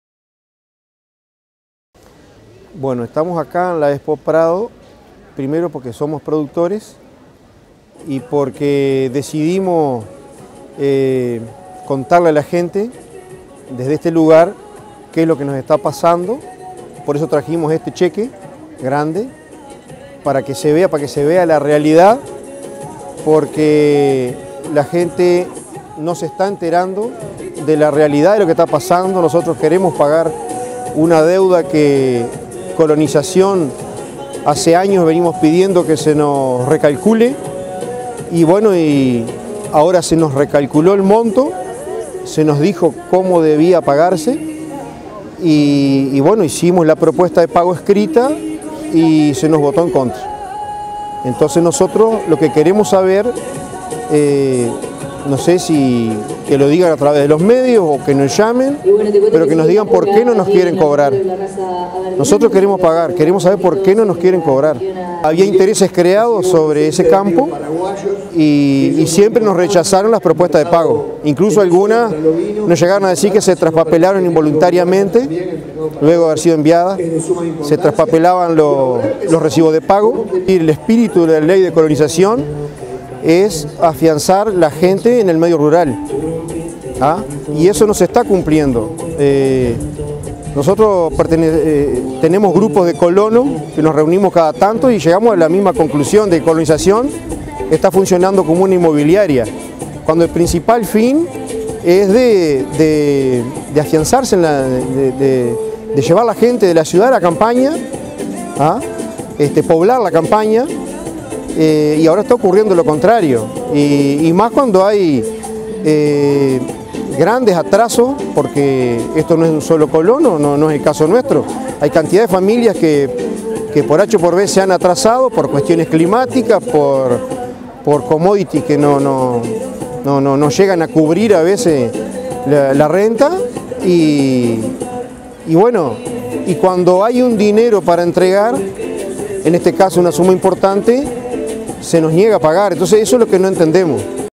Productores en la Expo Prado
Expresan que quieren pagar deuda de colonización. Se recalculó monto, ofrecieron pagar y les dijeron que no.